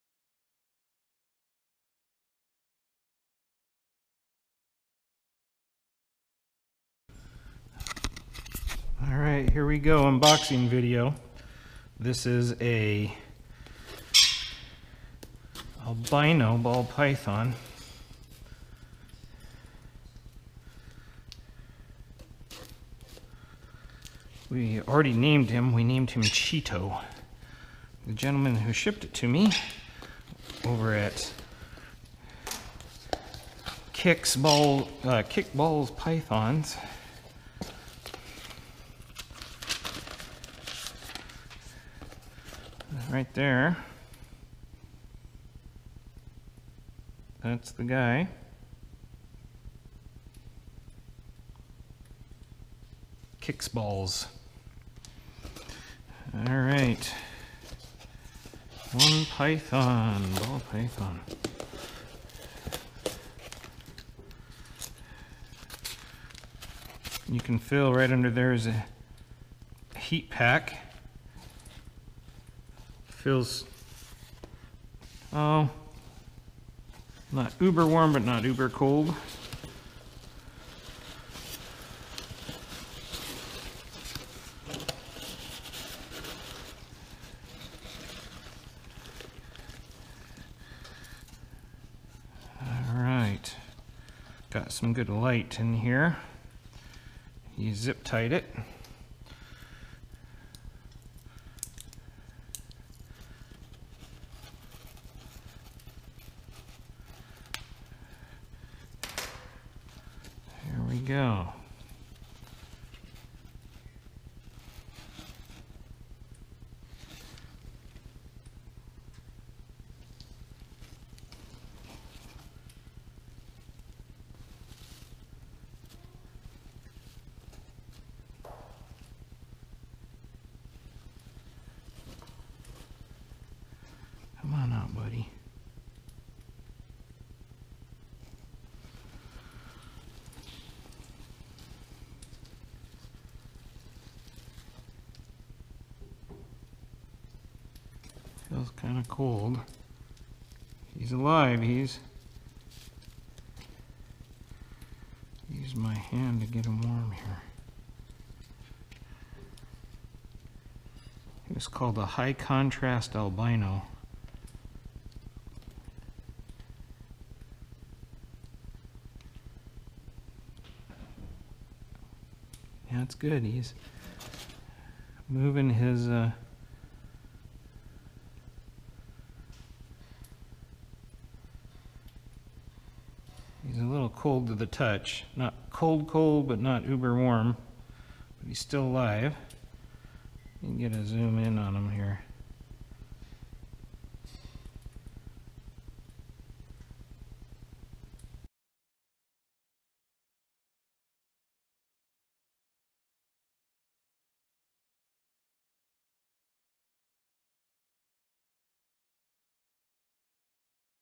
Cheeto unboxing video - Albino Ball Python
This is a video unboxing "Cheeto" the albino ball python.